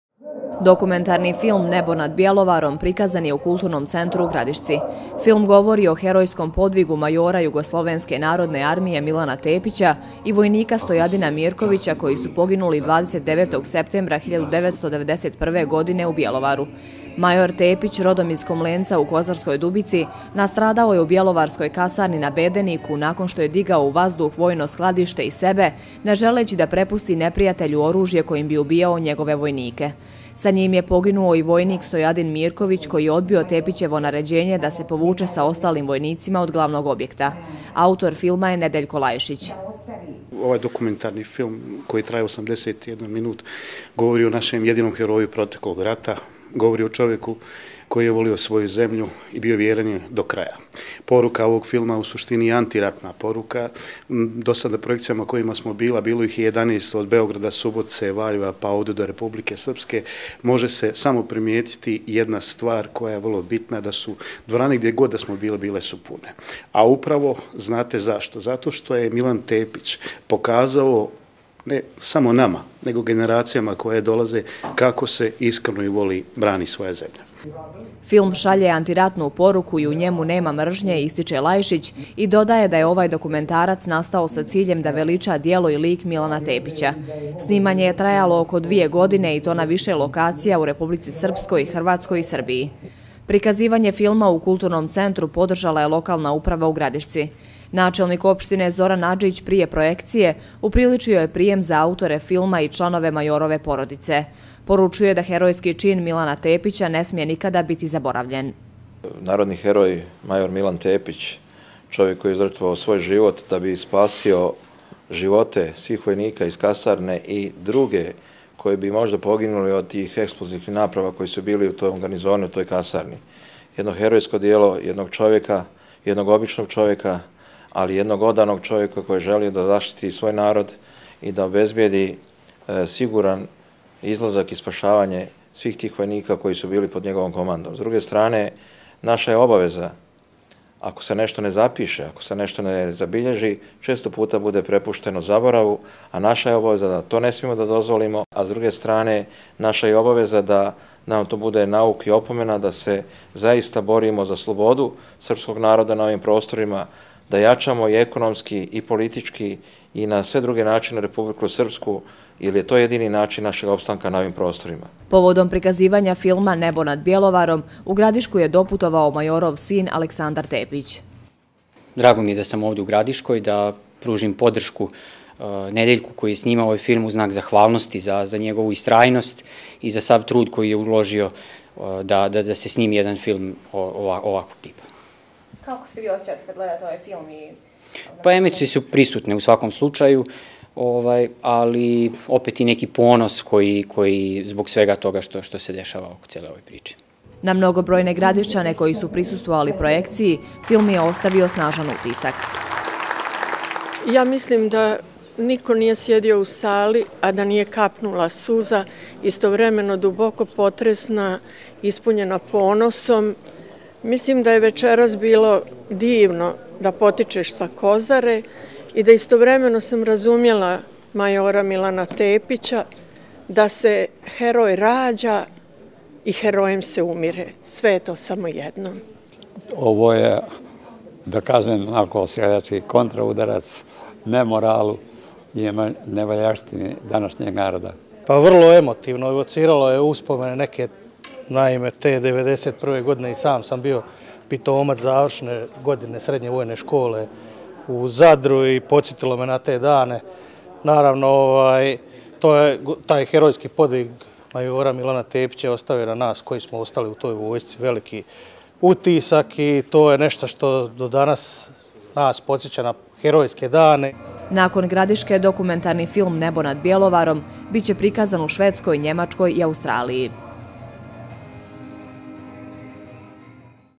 U Gradišci prikazan dokumentarni film „Nebo nad Bjelovarom“
Dokumentarni film „Nebo nad Bjelovarom“, prikazan u Kulturnom centru u Gradišci, govori o herojskom podvigu majora JNA Milana Tepića i vojnika Stojadina Mirkovića, koji su poginuli 29. septembra 1991. godine u Bjelovaru.